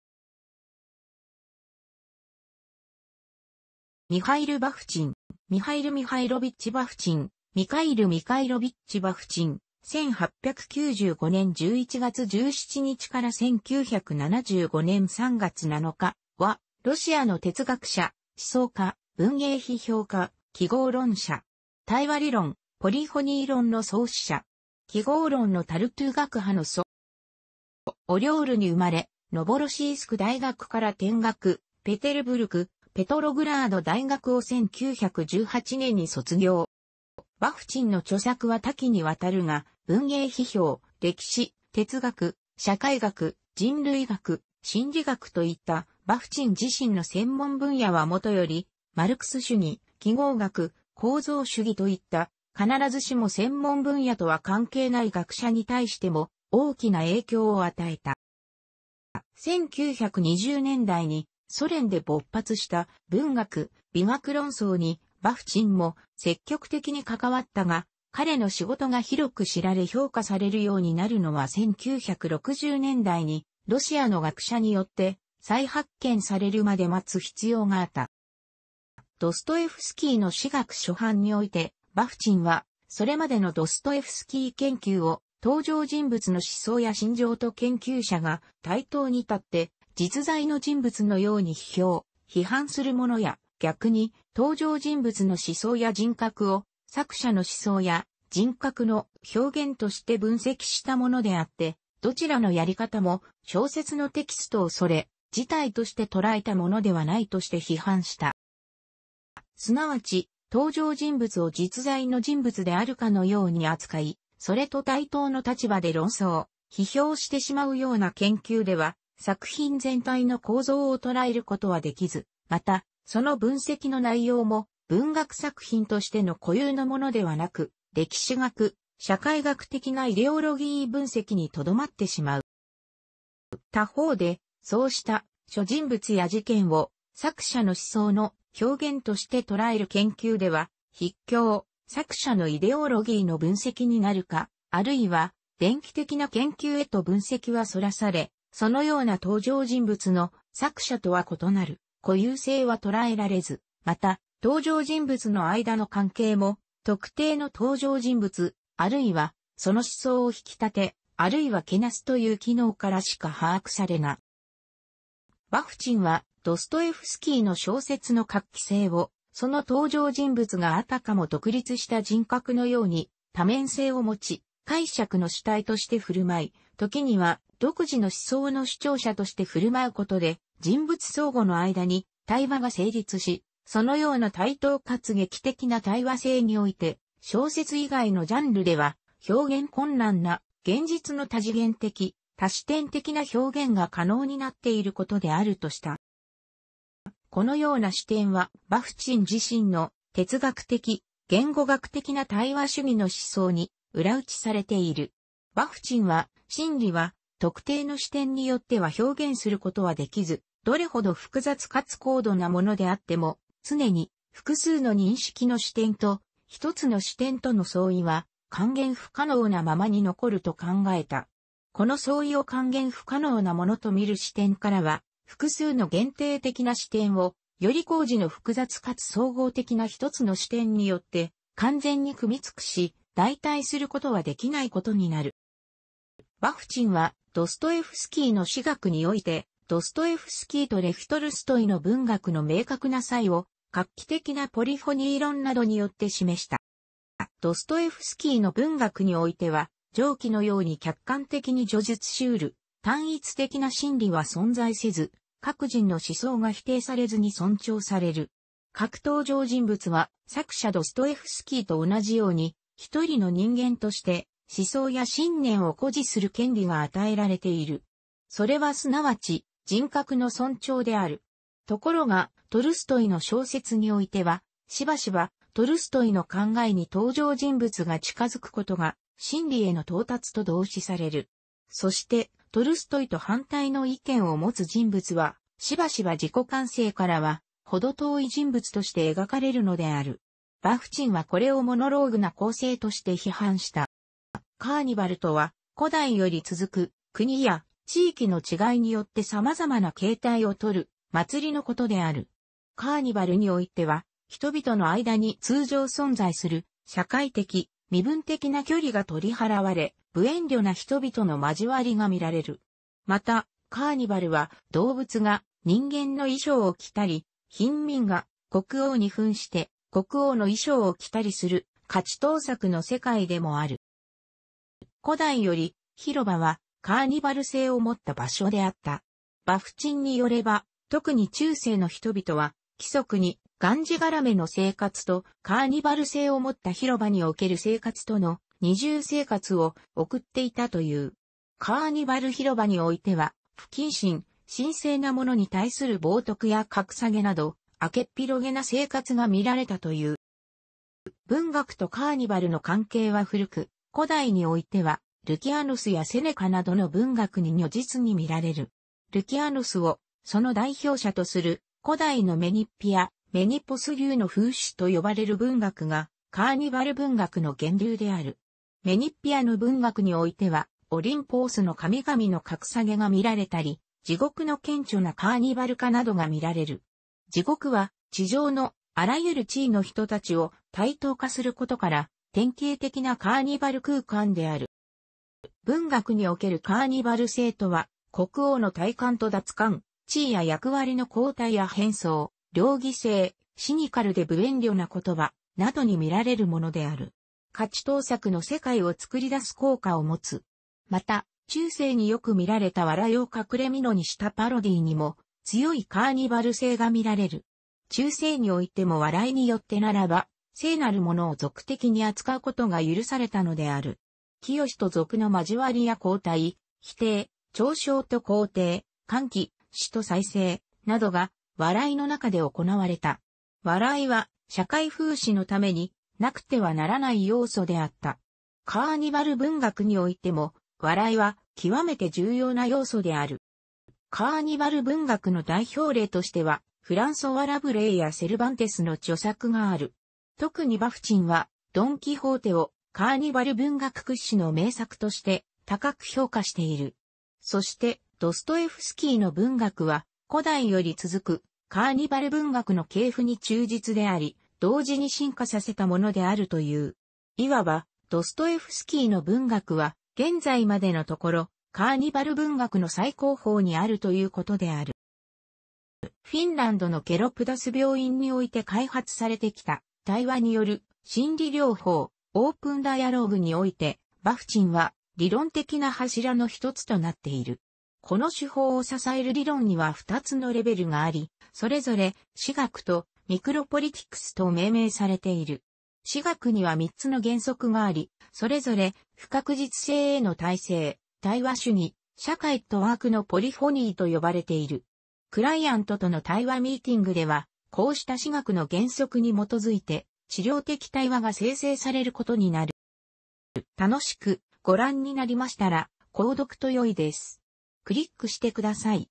ミハイル・バフチン
ミハイル・ミハイロビッチ・バフチン（Михаил Михайлович Бахти́н , Mikhail Mikhailovich Bakhtin,1895年11月17日 - 1975年3月7日）は、ロシアの哲学者、思想家、文芸批評家、記号論者。対話理論・ポリフォニー論の創始者。記号論のタルトゥー学派の祖。

オリョールに生まれ、ノヴォロシースク大学から転学、ペテルブルク（ペトログラード）大学を1918年に卒業。

バフチンの著作は多岐にわたるが、文芸批評、歴史、哲学、社会学、人類学、心理学といったバフチン自身の専門分野はもとより、マルクス主義、記号学、構造主義といった必ずしも専門分野とは関係ない学者に対しても大きな影響を与えた。

1920年代にソ連で勃発した文学・美学論争にバフチンも積極的に関わったが、彼の仕事が広く知られ評価されるようになるのは1960年代にロシアの学者によって再発見されるまで待つ必要があった。

「ドストエフスキーの詩学」初版において、バフチンは、それまでのドストエフスキー研究を、登場人物の思想や心情と研究者が対等にたって、実在の人物のように批評・批判するものや、逆に、登場人物の思想や人格を、作者の思想や人格の「表現」として分析したものであって、どちらのやり方も、小説のテキストをそれ自体として捉えたものではないとして批判した。

すなわち、登場人物を実在の人物であるかのように扱い、それと対等の立場で論争・批評してしまうような研究では、作品全体の構造を捉えることはできず、また、その分析の内容も、文学作品としての固有のものではなく、歴史学・社会学的なイデオロギー分析にとどまってしまう。

他方で、そうした諸人物や事件を、作者の思想の「表現」として捉える研究では、畢竟、作者のイデオロギーの分析になるか、あるいは、伝記的な研究へと分析は逸らされ、そのような登場人物の、作者とは異なる固有性は捉えられず、また、登場人物の間の関係も、特定の登場人物（あるいは、その思想）を引き立て、あるいはけなすという機能からしか把握されない。

バフチンはドストエフスキーの小説の画期性を、その登場人物があたかも独立した人格のように多面性を持ち、解釈の主体として振舞い、時には、独自の思想の主張者として振舞うことで、人物相互の間に「対話」が成立し、そのような対等かつ劇的な対話性において、小説以外のジャンルでは表現困難な、現実の多次元的・多視点的な表現が可能になっていることであるとした。

このような視点は、バフチン自身の、哲学的・言語学的な対話主義の思想に裏打ちされている。バフチンは、真理は、特定の視点によっては表現することはできず、どれほど複雑かつ高度なものであっても、つねに複数の認識の視点と、ひとつの視点との相違は還元不可能なままに残ると考えた。この相違を還元不可能なものと見る視点からは、複数の限定的な視点を、より高次の複雑かつ総合的なひとつの視点によって完全に汲みつくし、代替することはできないことになる。

バフチンは「ドストエフスキーの詩学」において、ドストエフスキーとレフ・トルストイの文学の明確な差異を、画期的なポリフォニー論などによって示した。

ドストエフスキーの文学においては、上記のように客観的に叙述し得る単一的な真理は存在せず、各人の思想が否定されずに尊重される。各登場人物は、作者ドストエフスキーと同じように、1人の人間として思想や信念を固持する権利が与えられている。それはすなわち人格の尊重である。ところがトルストイの小説においては、しばしばトルストイの考えに登場人物が近づくことが、真理への到達と同視される。そしてトルストイと反対の意見を持つ人物は、しばしば自己完成からは程遠い人物として描かれるのである。バフチンはこれをモノローグな構成として批判した。

カーニバルとは古代より続く、国や地域の違いによって様々な形態をとる祭りのことである。カーニバルにおいては、人々の間に通常存在する社会的、身分的な距離が取り払われ、無遠慮な人々の交わりが見られる。また、カーニバルは、動物が人間の衣装を着たり、貧民が国王に扮して国王の衣装を着たりする、価値倒錯の世界でもある。

古代より、広場はカーニバル性をもった場所であった。バフチンによ...